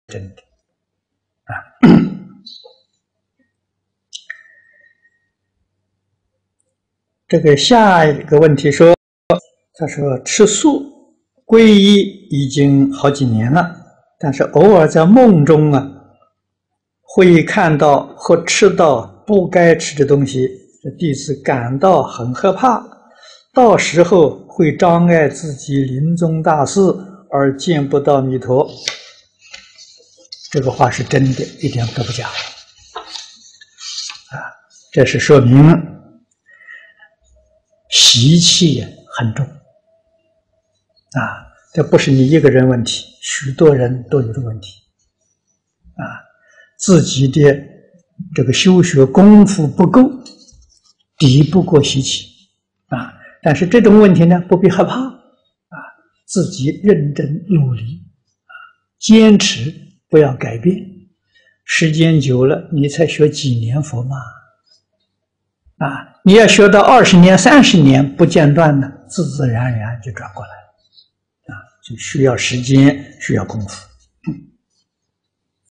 净空老法師:偶爾在夢中看到,吃到不該吃的東西,害怕到時會否障礙臨終大事?
影片字幕：
問：下一個問題，他說吃素、皈依已經好幾年了，但是偶爾在夢中會看到或吃到不該吃的東西，弟子感到很害怕，到時候會障礙自己臨終大事，而見不到彌陀。

答：這話是真的，一點都不假，這是說明習氣很重。這不是你一個人的問題，許多人都有這個問題，自己的修學功夫不夠，敵不過習氣。但是這種問題不必害怕，自己認真努力，堅持不要改變，時間久了，你才學幾年佛，你要學到二十年、三十年不間斷，自自然然就轉過來，所以需要時間，需要功夫。